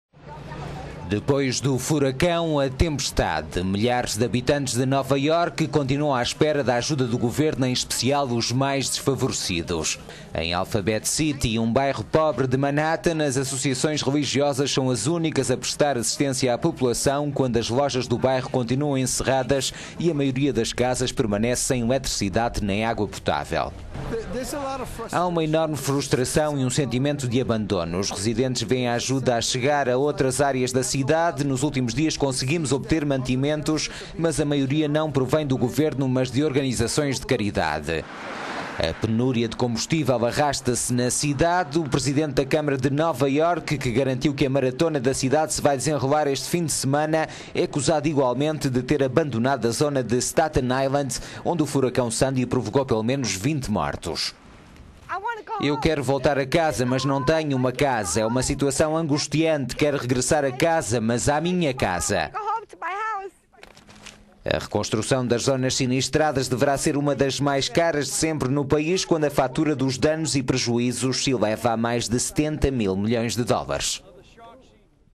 Sandy: a revolta dos "abandonados"
Depois do furacão, a tempestade. Milhares de habitantes de Nova Iorque continuam à espera da ajuda do governo, em especial os mais desfavorecidos.

Em "Alphabet City", um bairro pobre de Manhattan, as associações religiosas são as únicas a prestar assistência à população, quando as lojas do bairro continuam encerradas e a maioria das casas permanece sem eletricidade nem água potável.

Para o pastor da igreja local, Richard Del Rio:
"Há uma enorme frustração e um sentimento de abandono. Os residentes vêem a ajuda a chegar a outras áreas da cidade. Nos últimos dias conseguimos obter mantimentos, mas a maioria não provém do governo mas de organizações de caridade, igrejas, etc".

O presidente da câmara de Nova Iorque, que garantiu que a maratona da cidade se vai desenrolar este fim de semana, é acusado igualmente de ter abandonado a zona de Staten Island, onde o furacão Sandy provocou pelo menos 20 mortos.

Uma residente não esconde a revolta:

"Eu quero voltar a casa, mas não tenho uma casa para onde ir. É uma situação angustiante, quero regressar a casa, mas à minha casa".

As críticas à gestão da catástrofe ocorrem quando o número de vítimas ascende a 98 mortos, quase metade dos quais em Nova Iorque. A reconstrução das zonas sinistradas deverá ser uma das mais caras de sempre no país, quando a fatura dos danos e prejuízos se eleva a mais de 70 mil milhões de dólares.